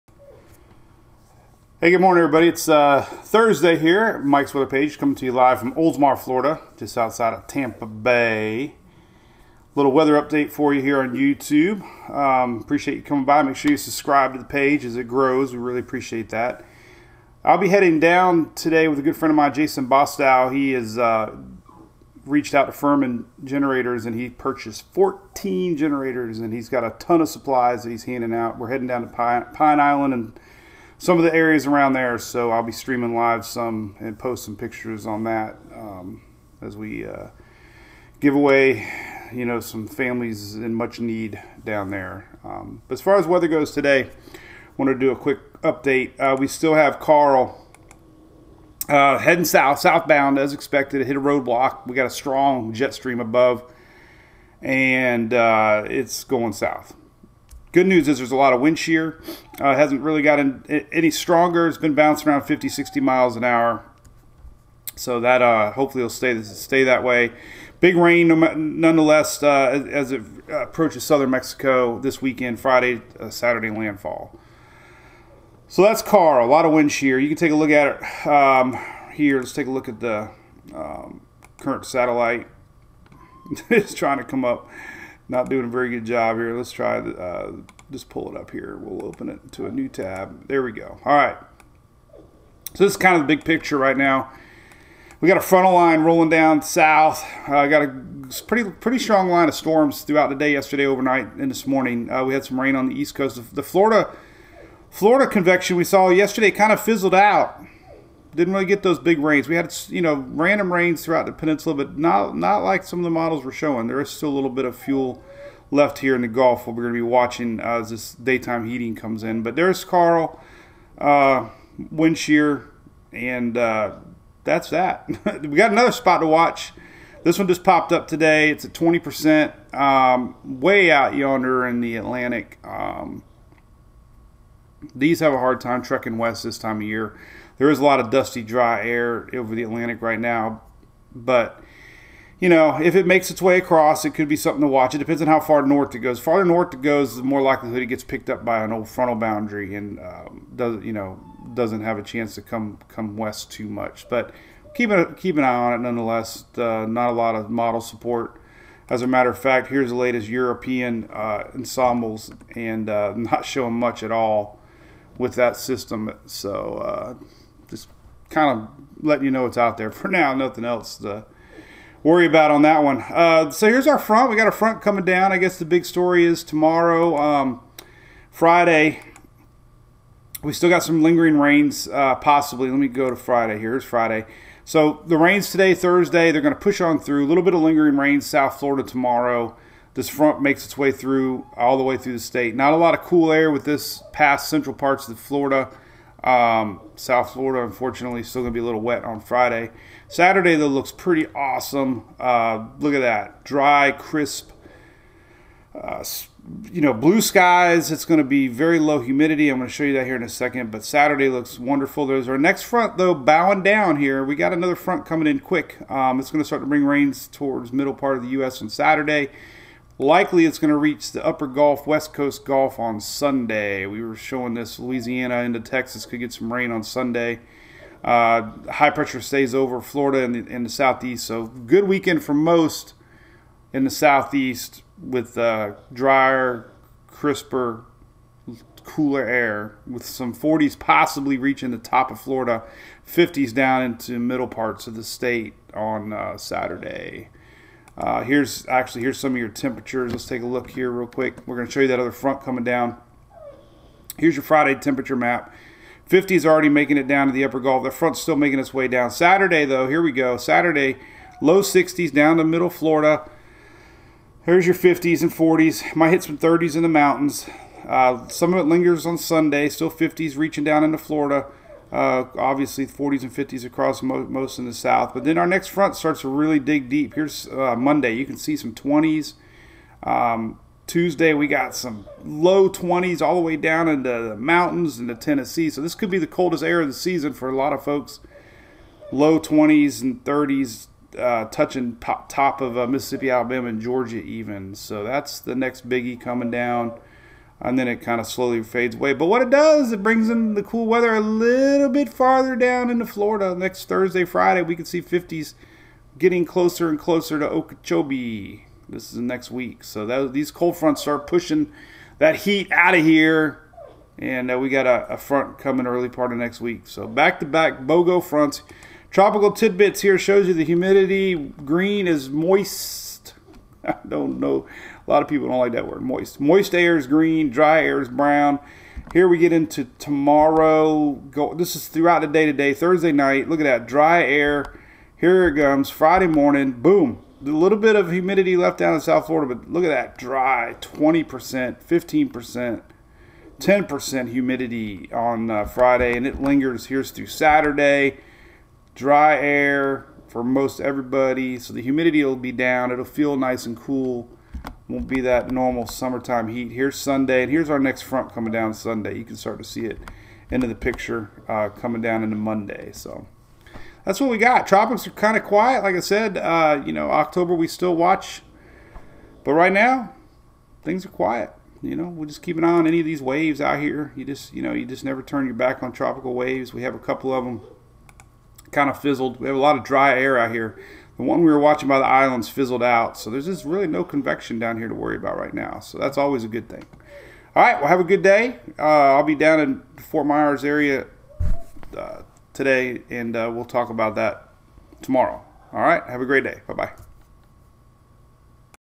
Karl heading southbound into Mexico and cold fronts are coming
Cold fronts are making their way down. Coldest air of the season next week for many. Karl heading south. New yellow spot out in the Atlantic.